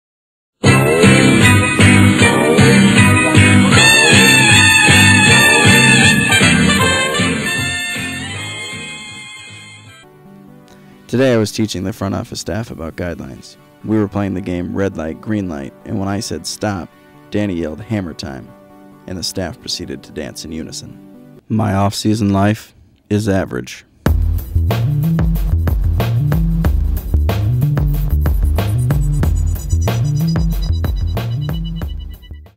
Episode #2 - Red Light
Casey tries teaching the Omaha Royals front office staff, but discovers the offseason has made most of them a bit crazy already!